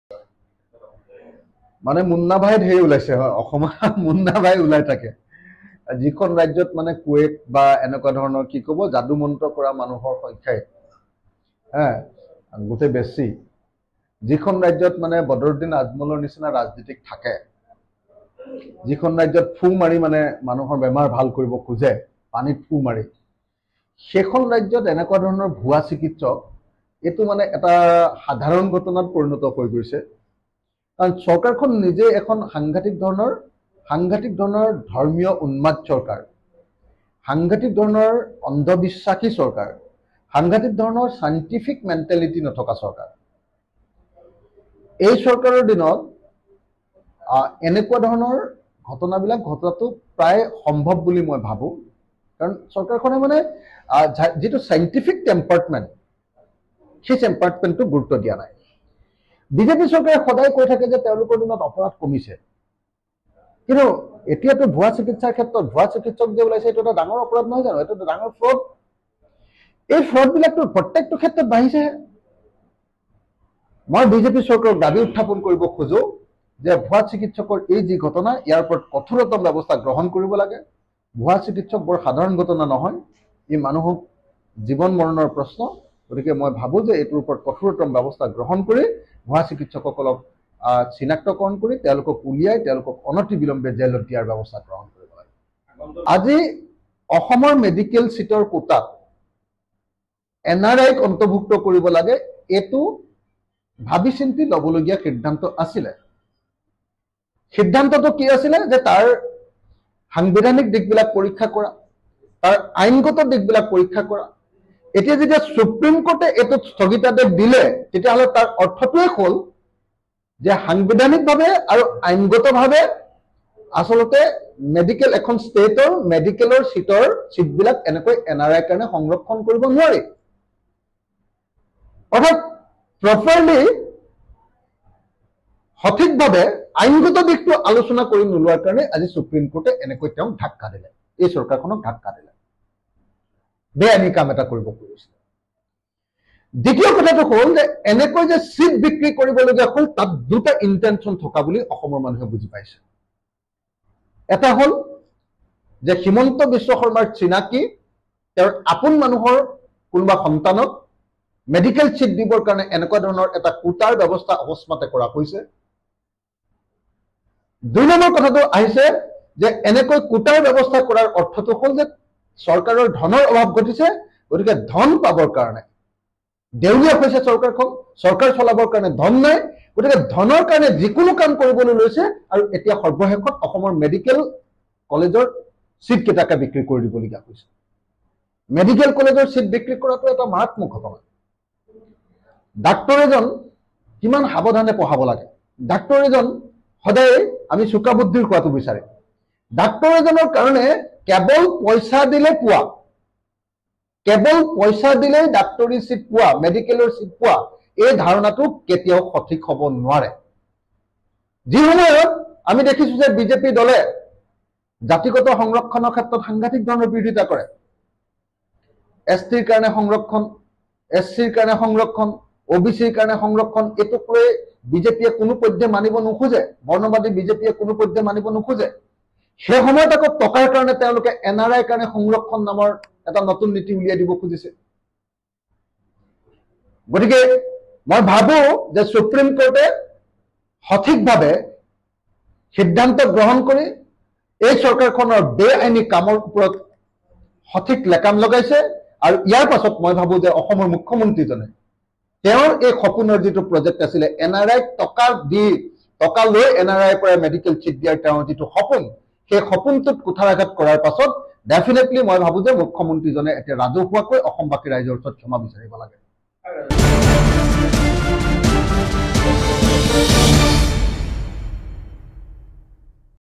ভুৱা চিকিৎসকৰ ওপৰত প্ৰতিক্ৰিয়া অখিল গগৈৰ
-'যিখন ৰাজ্যত ফু মাৰি বেমাৰ ভাল কৰা মানুহ থাকে
-সেইখন ৰাজ্যত এনে ভুৱা চিকিৎসক থকাটো এটা সাধাৰণ ঘটনাত পৰিণত হৈছে'
-'এই চৰকাৰ ধৰ্ম উন্মাদ চৰকাৰ, অন্ধবিশ্বাসী চৰকাৰ'
-'ভুৱা চিকিৎসকতো এটা ডাঙৰ অপৰাধ'
-'ইয়াৰ বিৰুদ্ধে কঠোৰ ব্যৱস্থা গ্ৰহণ কৰাৰ দাবী'